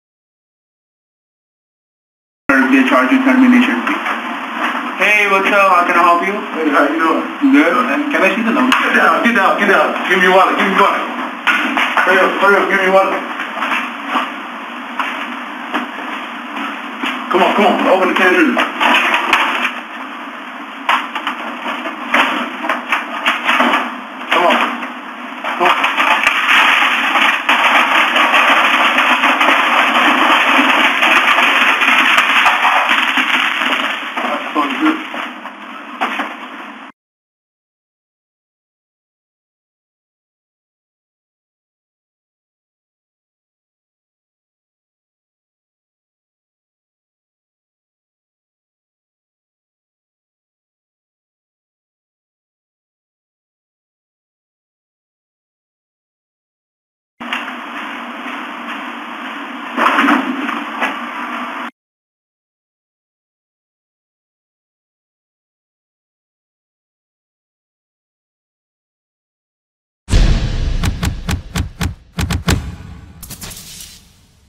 Robber Pistol-Whips Customer During Robbery in Houston
ON THE LOOSE - Houston Police released footage of a robber entering an AT&T store, hitting the back of a customer's head with his pistol, and forcing an employee to hand over cash and cell-phones. As of now, the suspect is still on the loose.